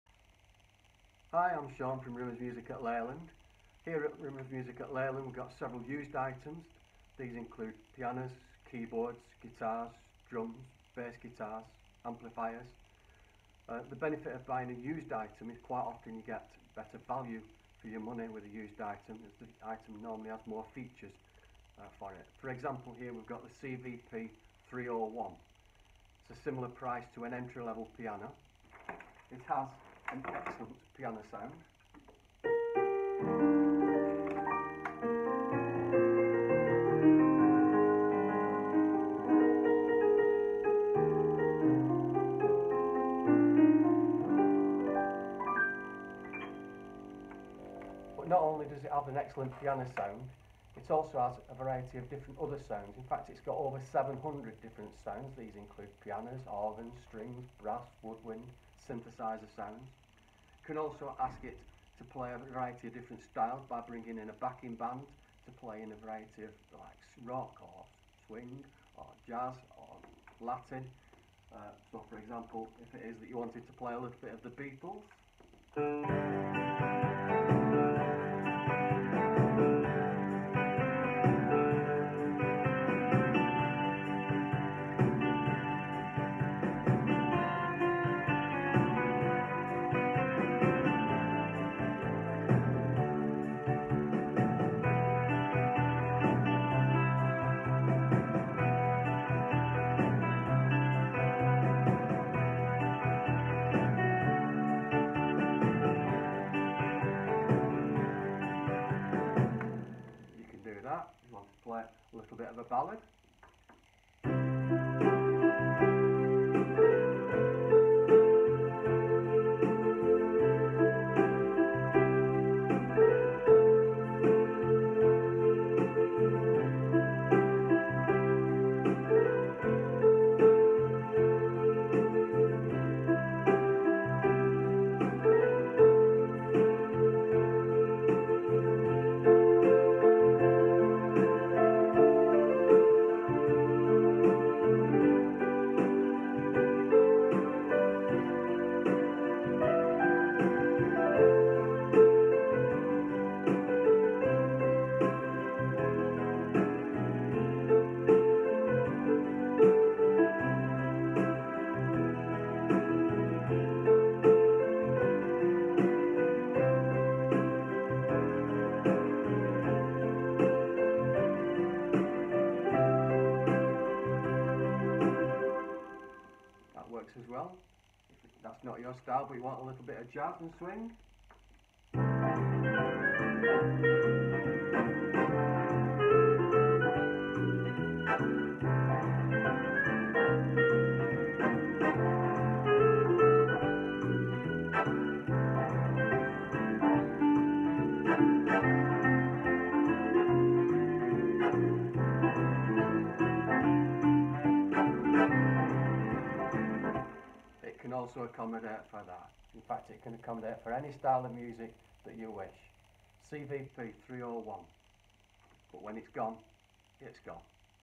Yamaha CVP 701 USED - Rimmers Music
Here we have Shaun over in our Leyland Store, showing you a fantastic USED Yamaha CVP301. We have an extensive range of used instruments in our store, that all provide you with excellent value for money.